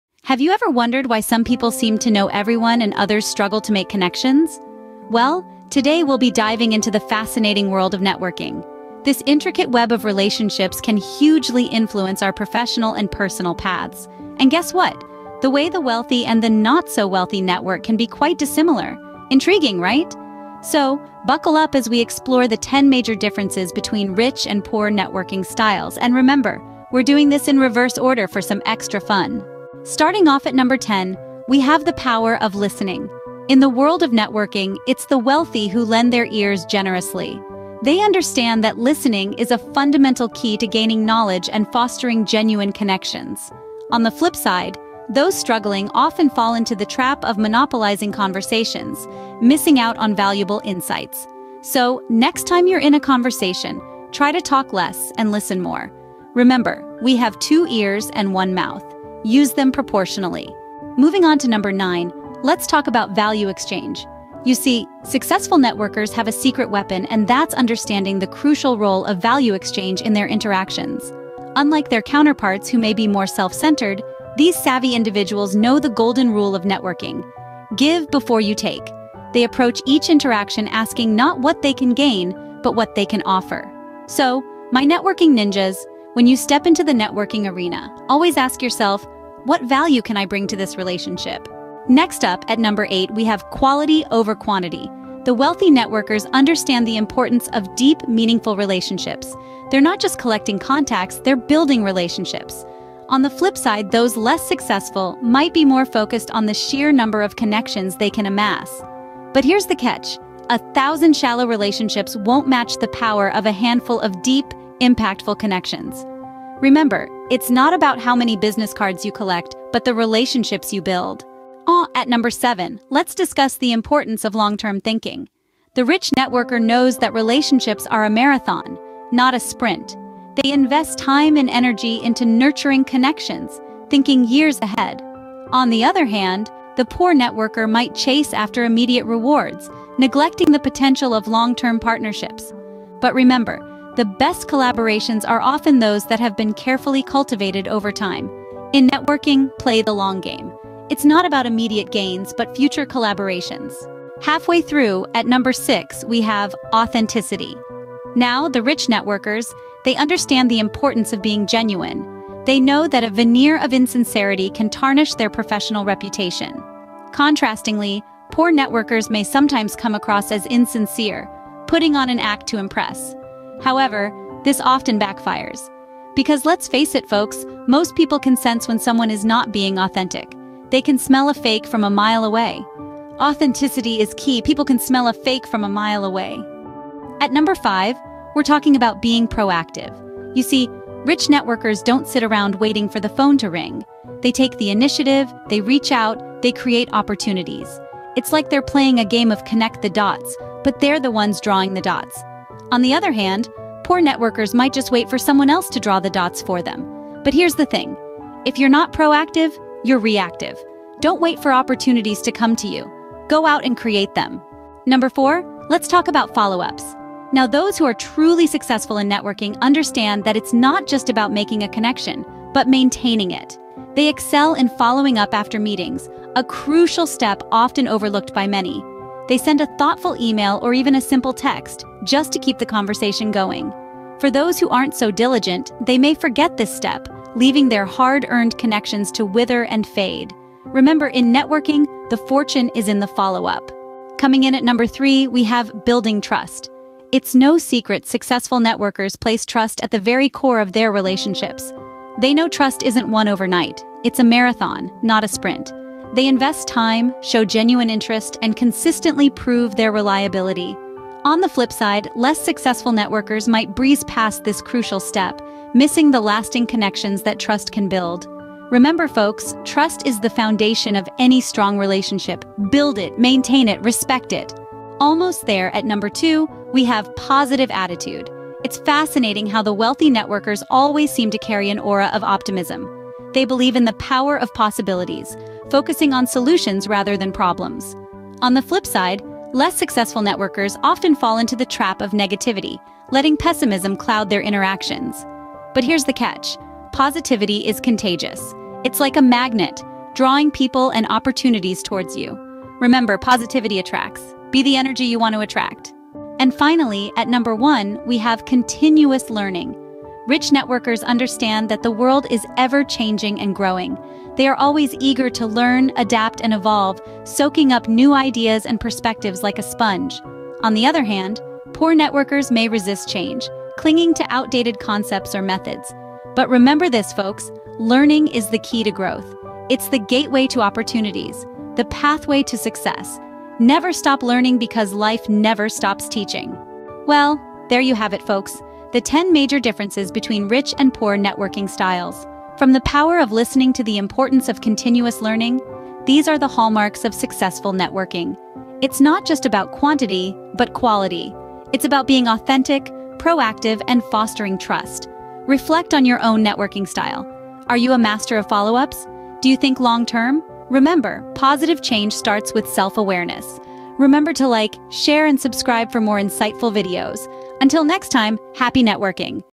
Top 10 Rich vs. Poor Networking Styles!
Welcome to our illuminating discussion on the top 10 Rich vs. Poor Networking Styles! In this intriguing video, we delve into the distinct approaches to networking employed by individuals from different socioeconomic backgrounds. From exclusive clubs to community gatherings, we explore how the wealthy and the less fortunate navigate social connections. Gain valuable insights into the strategies and nuances that shape networking dynamics in various economic spheres. Join us as we dissect these contrasting styles and uncover valuable lessons for building meaningful connections in any context. Discover the secrets to effective networking and bridge the gap between privilege and opportunity.

OUTLINE:
00:00:00 Introduction
00:00:29 The Power of Listening
00:01:00 Value Exchange
00:01:31 Quality over Quantity
00:02:01 Long-term Thinking
00:02:33 Authenticity
00:03:06 Proactive Networking
00:03:34 Master of Follow-ups
00:04:03 Building Trust
00:04:34 Positive Attitude
00:05:06 Continuous Learning
00:05:36 Conclusion